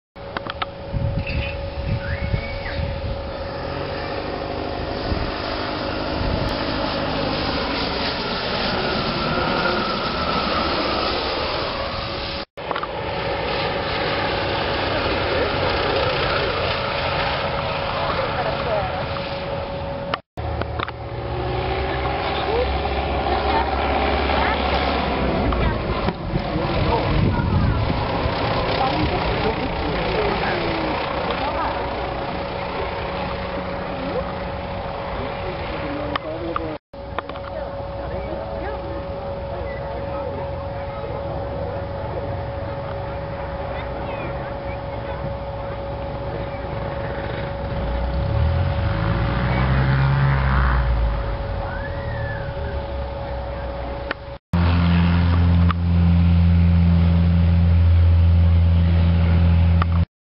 Glider towing at the Keiheuvel Cup 2009
Glider towing aircraft at the Keiheuvel Cup 2009. Christen A-1 Husky (OO-AZM), PA-25 Pawnee (PH-BJX) and American Champion 8GCBC Scout (OO-KMT).